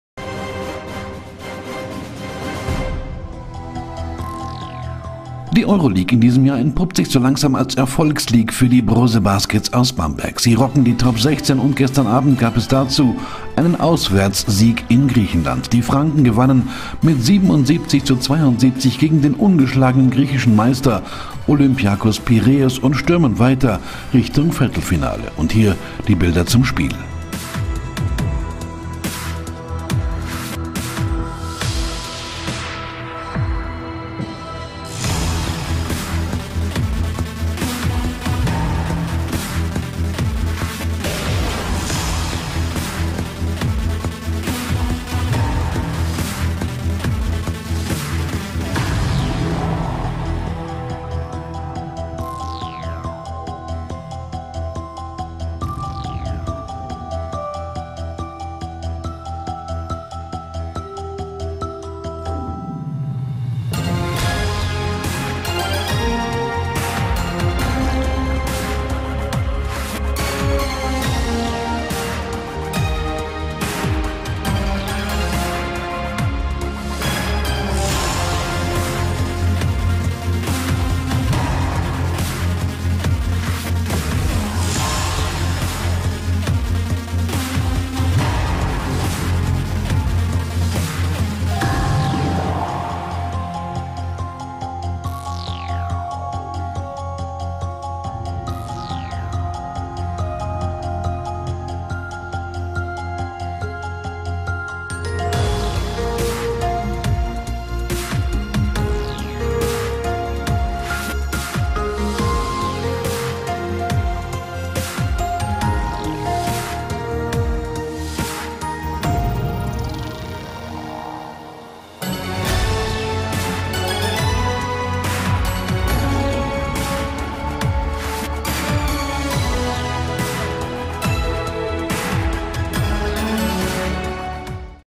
Basketball EL: Brose Baskets rocken Top 16
Die Euroleague in diesem Jahr entpuppt sich so langsam als Erfolgsleague für die Brose Baskets aus Bamberg. Sie siegen weiter und gestern Abend gab es ein 77:72 gegen Olympiakos Piräus.